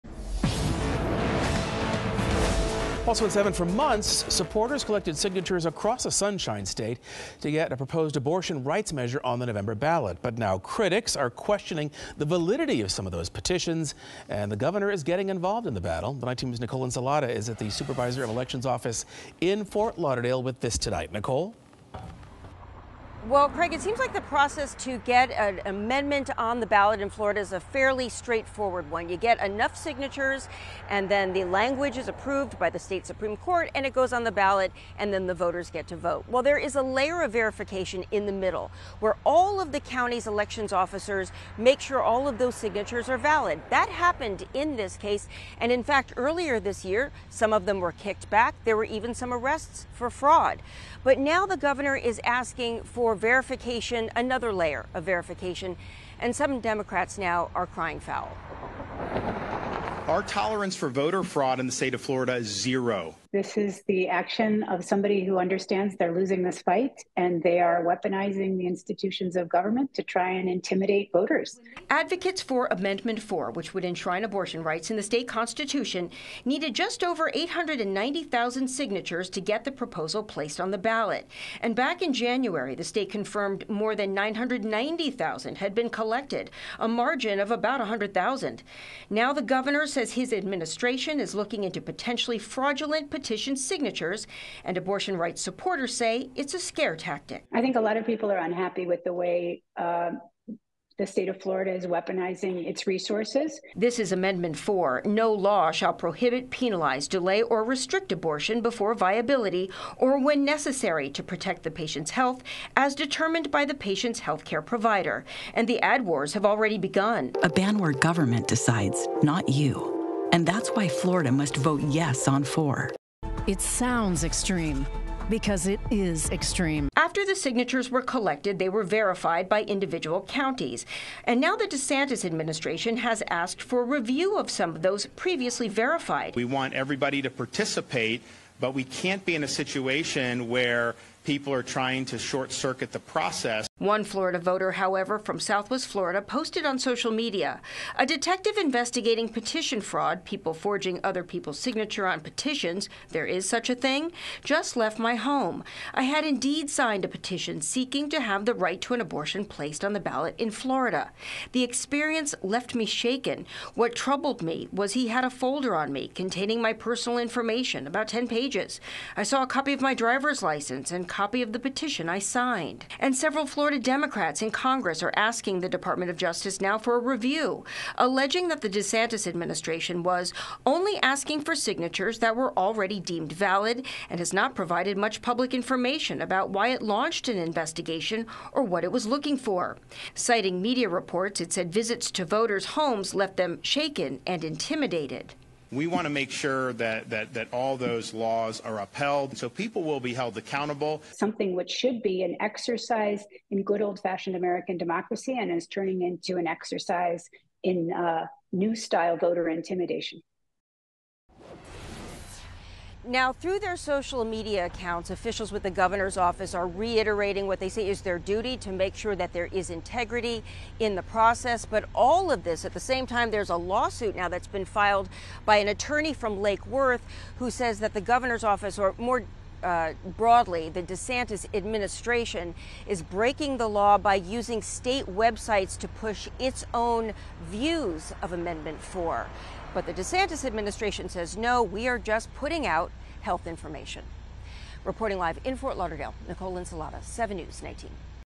Police are questioning Florida voters about signing an abortion rights ballot petition
State police are showing up at Florida voters’ homes to question them about signing a petition to get an abortion rights amendment on the ballot in November, and a state health care agency has launched a website targeting the ballot initiative with politically charged language.